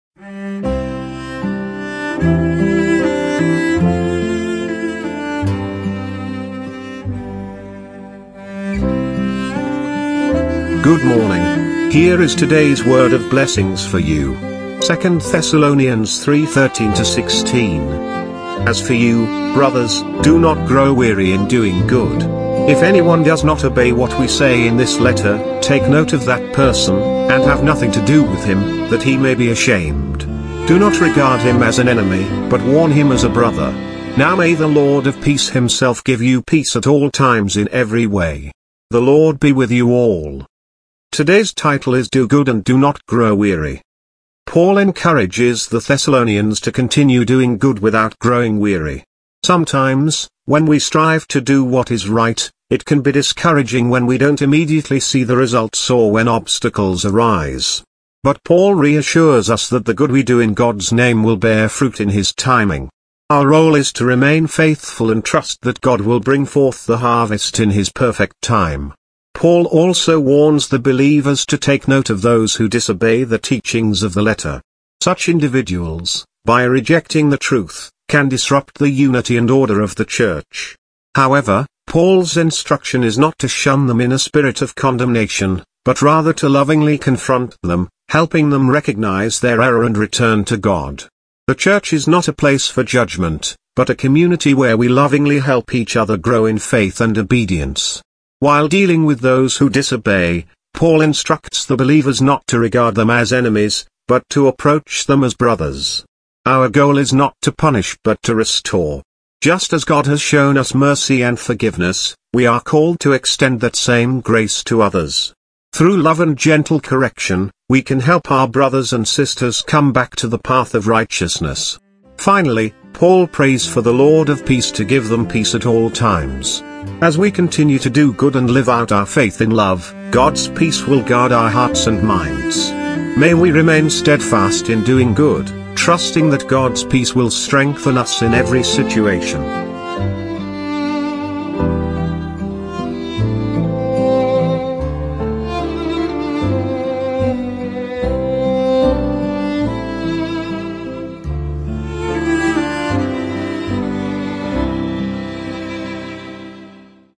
Do Good and Do Not Grow Weary (2 Thessalonians 3:13-16) Mission Blessings
Good Morning! Here is today’s word of blessings for you. 2 Thessalonians 3:13-16.

13 As for you, brothers, do not grow weary in doing good. 14 If anyone does not obey what we say in this letter, take note of that person, and have nothing to do with him, that he may be ashamed. 15 Do not regard him as an enemy, but warn him as a brother. 16 Now may the Lord of peace himself give you peace at all times in every way. The Lord be with you all.

Today’s title is **Do Good and Do Not Grow Weary**

Paul encourages the Thessalonians to continue doing good without growing weary. Sometimes, when we strive to do what is right, it can be discouraging when we don’t immediately see the results or when obstacles arise. But Paul reassures us that the good we do in God’s name will bear fruit in His timing. Our role is to remain faithful and trust that God will bring forth the harvest in His perfect time.

Paul also warns the believers to take note of those who disobey the teachings of the letter. Such individuals, by rejecting the truth, can disrupt the unity and order of the church. However, Paul’s instruction is not to shun them in a spirit of condemnation, but rather to lovingly confront them, helping them recognize their error and return to God. The church is not a place for judgment, but a community where we lovingly help each other grow in faith and obedience.

While dealing with those who disobey, Paul instructs the believers not to regard them as enemies, but to approach them as brothers. Our goal is not to punish but to restore. Just as God has shown us mercy and forgiveness, we are called to extend that same grace to others. Through love and gentle correction, we can help our brothers and sisters come back to the path of righteousness.

Finally, Paul prays for the Lord of peace to give them peace at all times. As we continue to do good and live out our faith in love, God’s peace will guard our hearts and minds. May we remain steadfast in doing good, trusting that God’s peace will strengthen us in every situation.